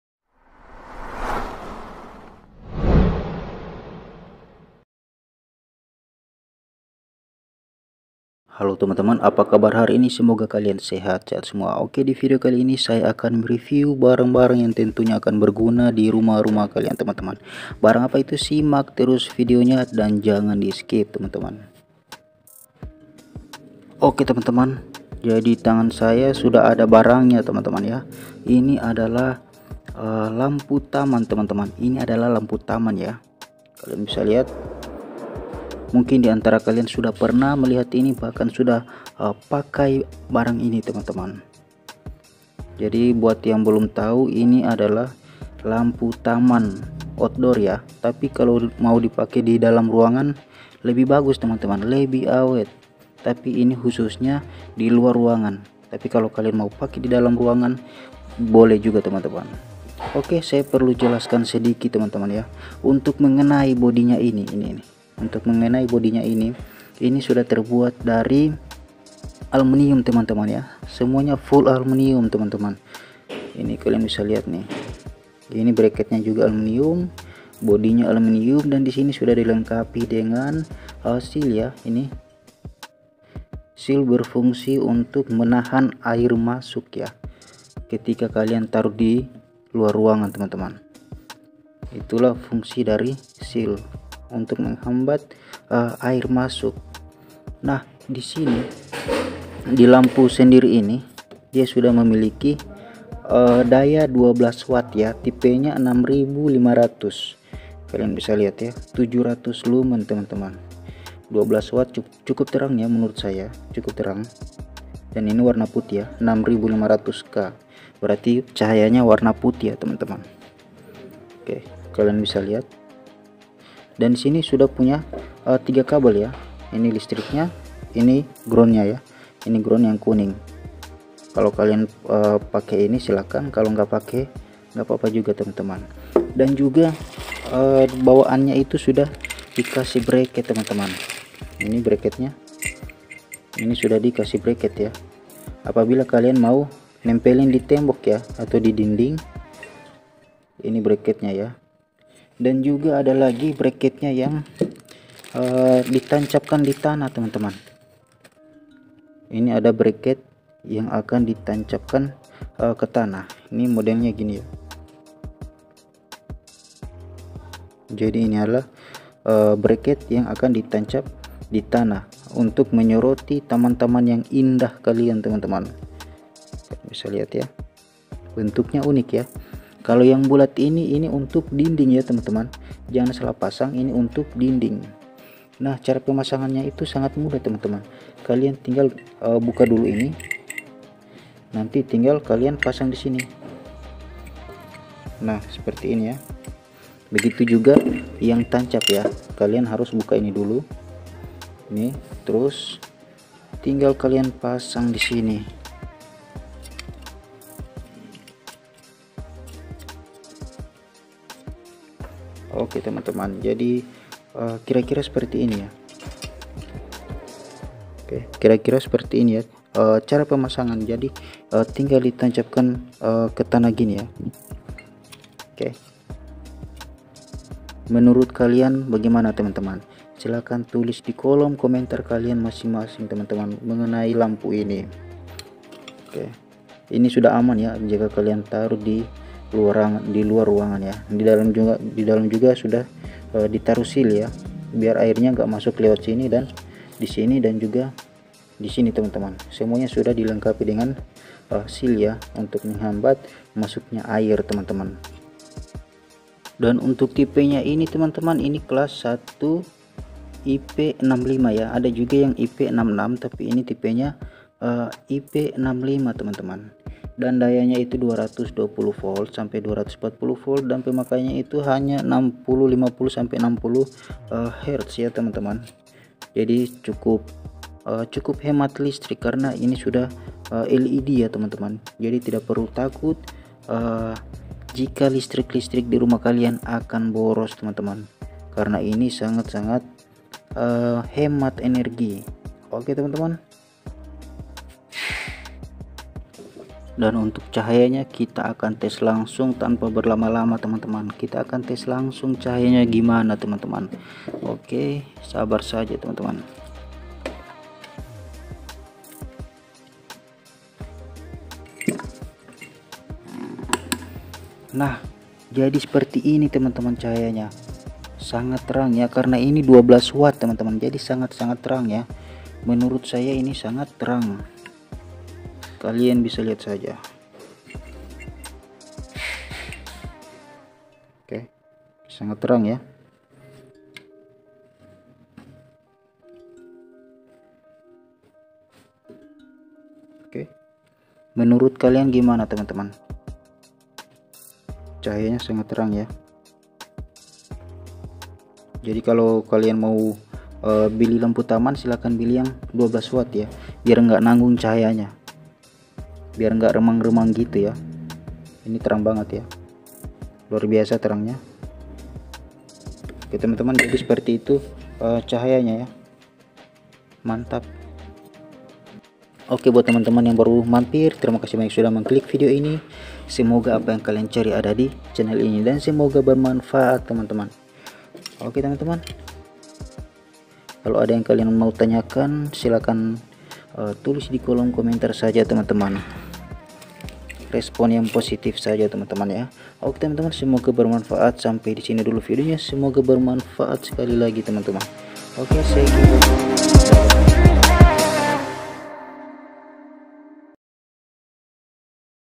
Review Lampu Sorot taman outdoor 12 watt
halo guys apa kabar hari ini semoga kalian sehat semua.oke di video kali ini saya akan me reviewe barang lagi.nah di video kali ini saya  akan membahas lampu sorot taman 12 watt..semoga saja bermanfaat dan semoga kalian ingin membeli barang ini amiinn.ok buat kalian simak saja videonya dan jangan di skip.enjooyy..

murah meriah..

lumayan murah...

hmm.